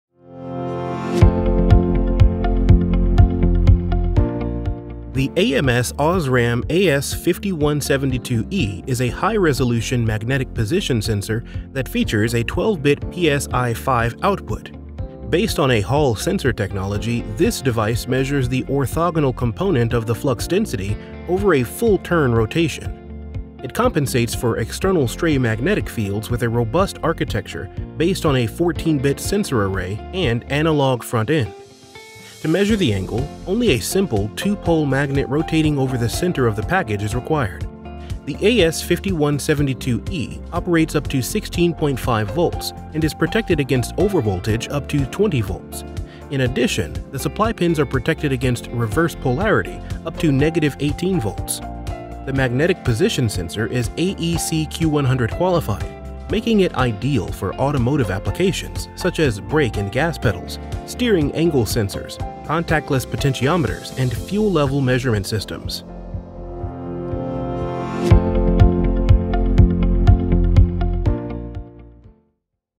ams OSRAM AS5172E High-Resolution Magnetic Position Sensor | New Product Brief
The ams OSRAM AS5172E is a High-Resolution Magnetic Position Sensor that features a 12-bit PSI5 output.